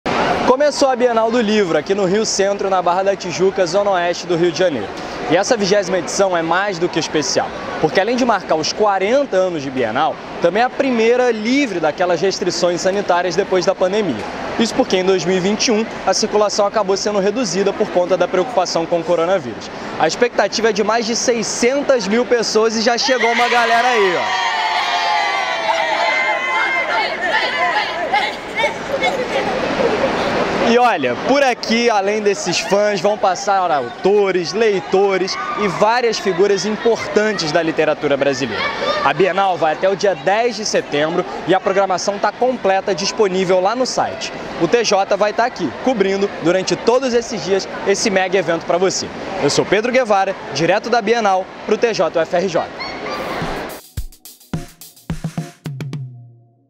O TJ UFRJ já chegou na Bienal do Livro
O TJ UFRJ já chegou na Bienal do Livro, na zona oeste do Rio de Janeiro! 📚
A partir desta sexta-feira, 01/09, vamos mostrar um pouco de tudo o que vem rolando no Rio Centro.
O evento ocorre até o dia 10 de setembro e reune muitos autores, estudantes e histórias.